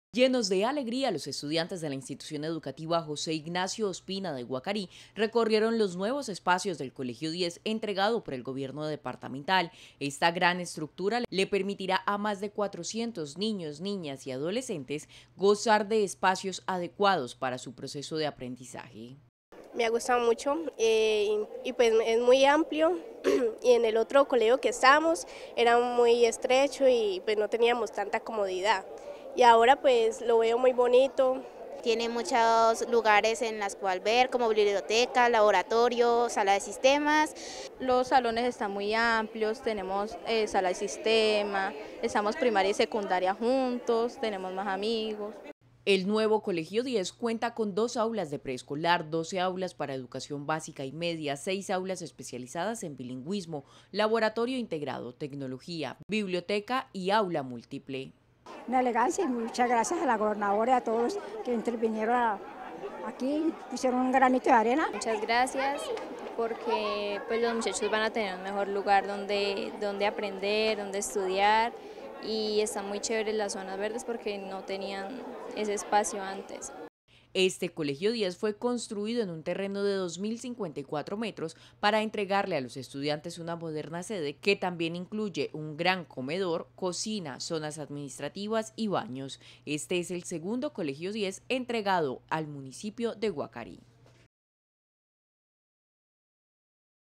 Así celebraron los niños y jóvenes de Guacarí la apertura del nuevo
Emitido el 24  de   enero 2023 día  por el Canal regional Telepacífico.

Con alegría y la emoción de ver hecho realidad el nuevo 'Colegio 10' los guacariseños celebraron la apertura de la institución educativa que le brindará a más de 400 estudiantes, la posibilidad de aprender a través del uso de moderna tecnología.